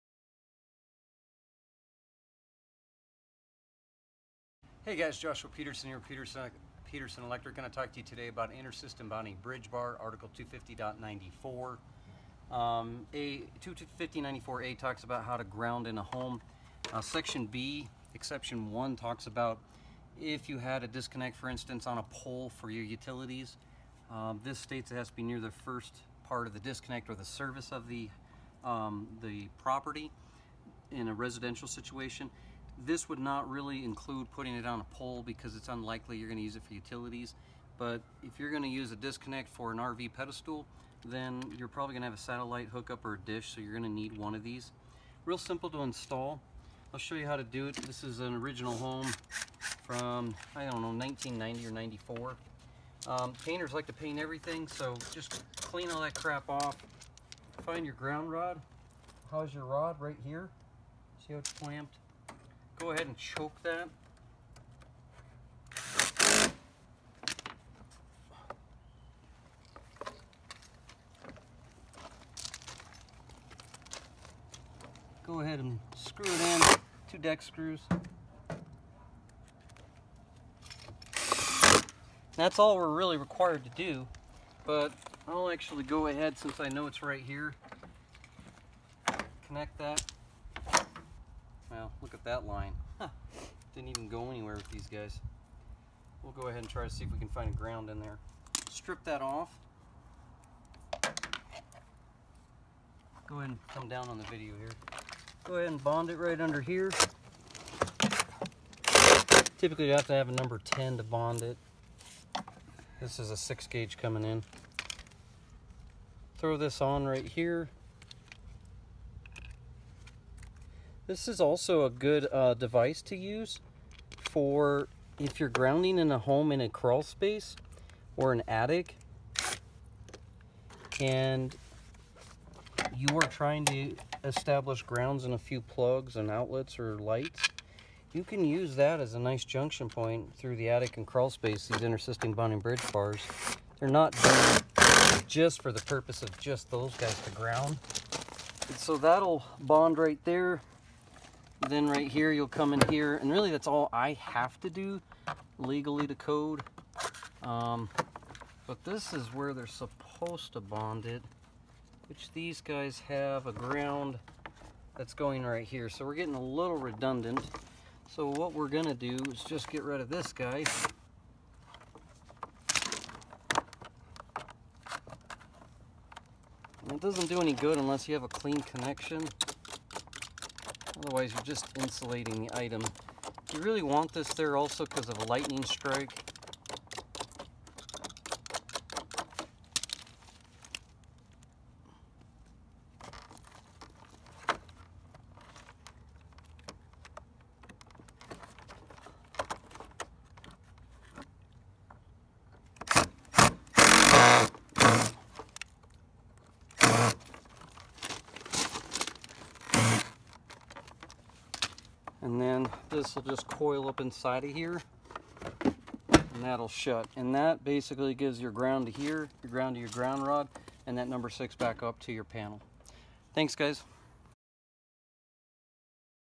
Intersystem Bonding Bar (IBTB) For Homeowner's Low Voltage Grounding
A customer had us install a new 100 amp panel in the basement. We had to update the grounding for the whole house. We had to install an intersystem bonding bar and make sure there was a cold water bond to the main water shut-off valve.